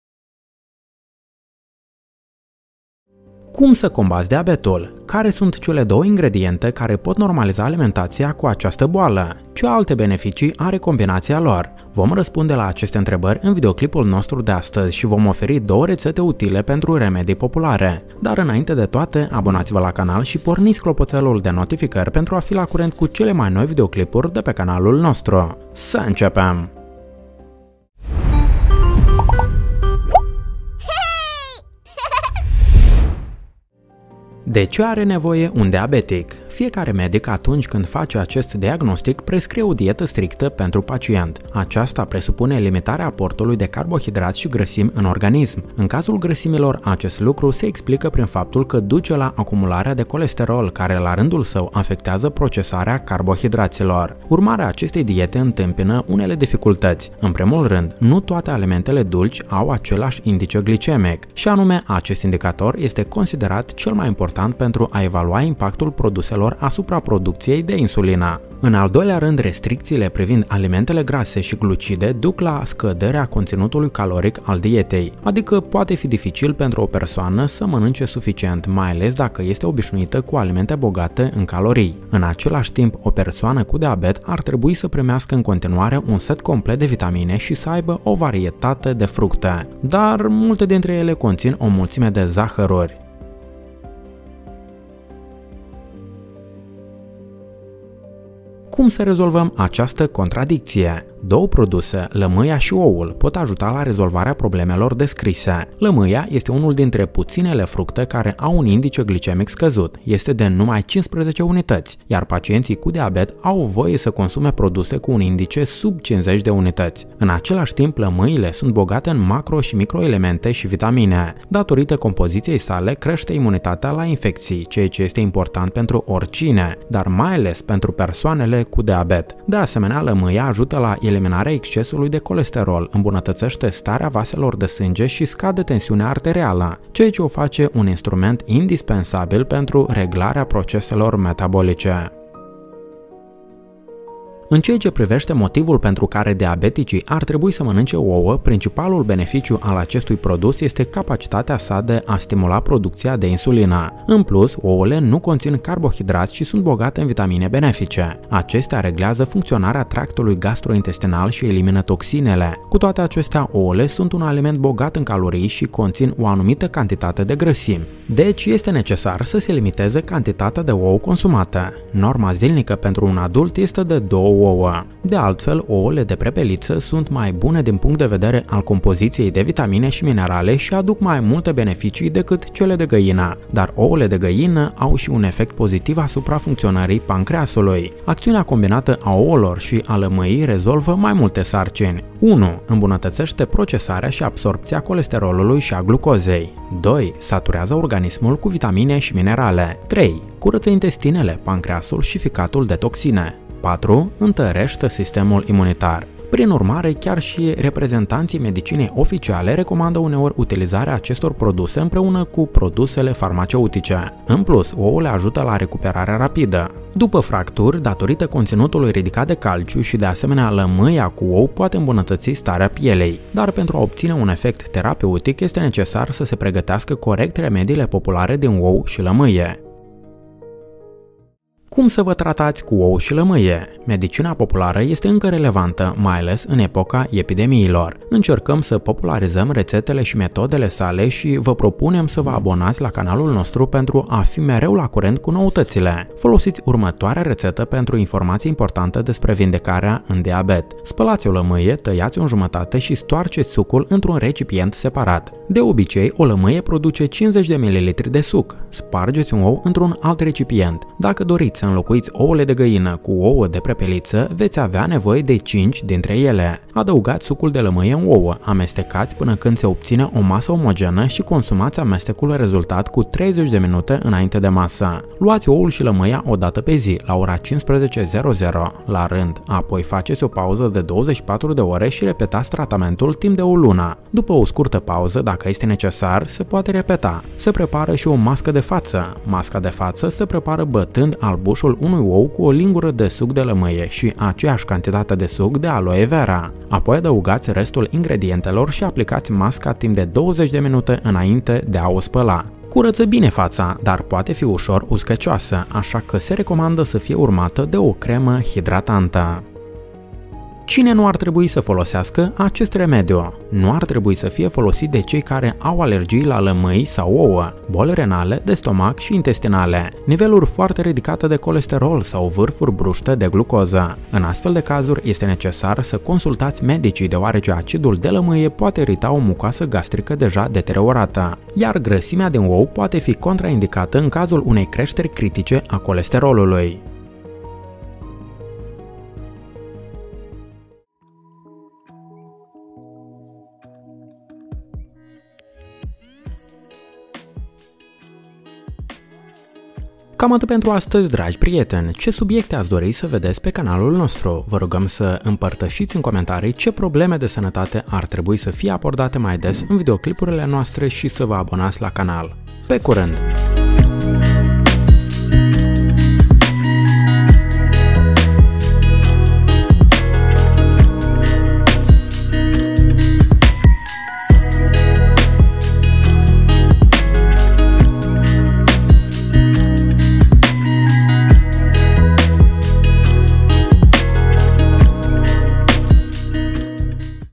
Tratează și Previne! Bea asta în fiecare Dimineață! Scade zahărul fără medicamente! DIABET
Surse De Inspiratie: Google, Wikipedia, Youtube, Facebook, OK.